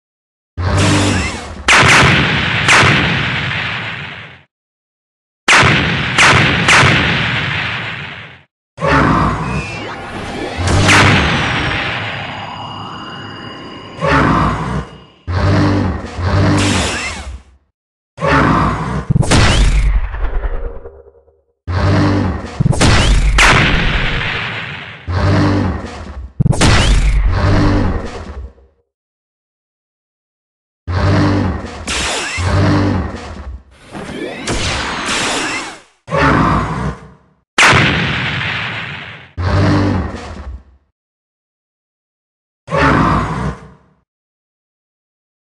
Godzilla 2019 vs shin godzilla
Hola chicos es mi segunda animacion en stick nodes y no es para un publico infantil